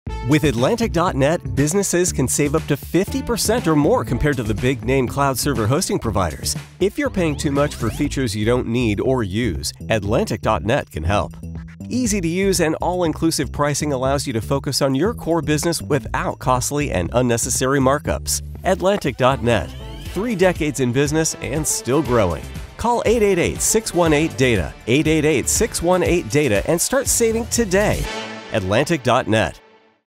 Atlantic.Net - Save Up to 50% on Cloud Server Hosting Compared to the Big Guys
With Atlantic.Net, businesses can save up to 50% or more compared to the big-name cloud server hosting providers. If you're paying too much for features you don't need or use, Atlantic.Net can help. Easy-to-use and all-inclusive pricing allows you to focus on your core business without costly and unnecessary markups. Atlantic.Net: 3 decades in business and still growing!